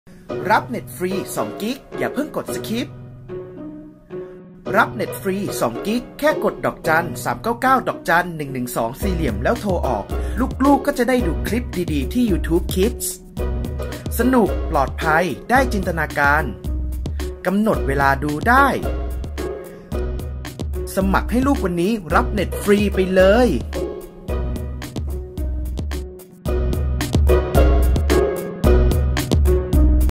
ดีแทคแจกเน็ต 2GB ให้ดู YouTube Kids ฟรี! ถึง11เม.ย.นี้เท่านั้น
ลูกค้าดีแทค รับเน็ต2GB/คน/สิทธิ์ สำหรับดูYouTube Kids ภายในวันที่ 11 เม.ย. 62 เท่านั้น
กด *399*112# โทรออก